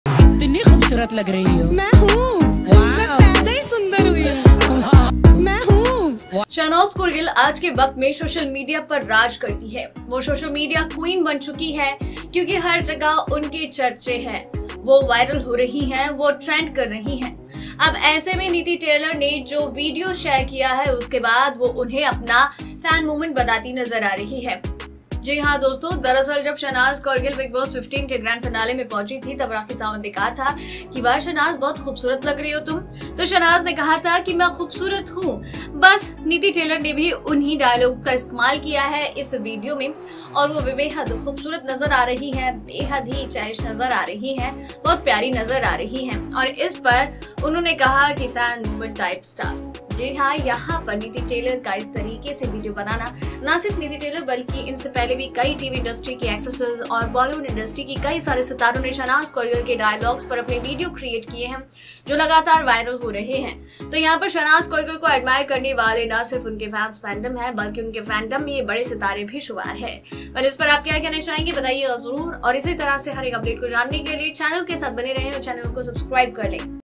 इस Actress ने Shehnaaz Gill पर यू बरसाया प्यार !
Bollywood Planet ( BP ) : Tv Actress Niti Taylor Shows Her Love For Shehnaaz Gill | Shehnaaz Gill News & Updates